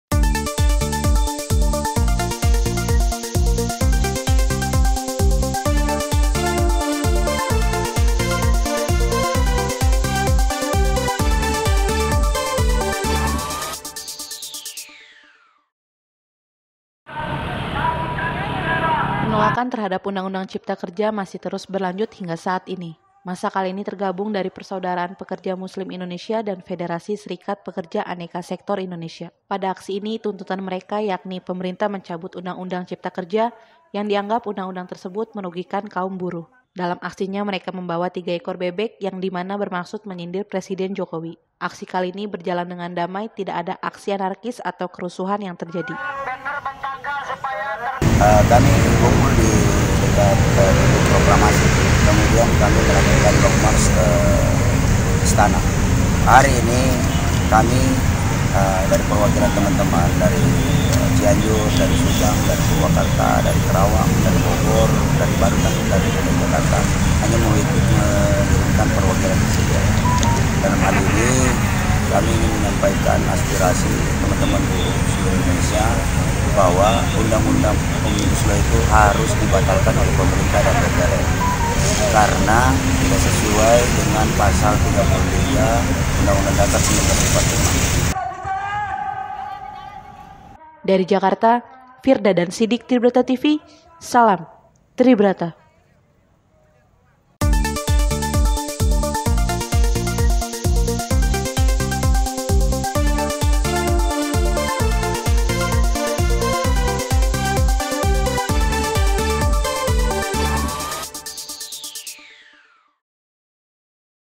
KRITIK JOKOWI, AKSI PENOLAKAN UU CIPTA KERJA MEMBAWA BEBEK
Penolakan terhadap UU cipta kerja masih terus berlanjut hingga saat ini. Massa kali ini tergabung dari persaudaraan pekerja muslim Indonesia, dan Federasi Serikat pekerja aneka sektor Indonesia.

pada aksi ini, tuntutan mereka yakni, pemerintah mencabut Undang-undang cipta kerja, yang dianggap Undang-undang tersebut merugikan kaum buruh.

Dalam aksinya mereka membawa 3 ekor bebek, yang dimana bermaksud menyindir Presiden Jokowi.

Aksi kali ini berjalan dengan damai, tidak ada aksi anarkis, atau kerusuhan yang terjadi.

Tim Liputan :  Virdha dan Sidik

Salam Tribrata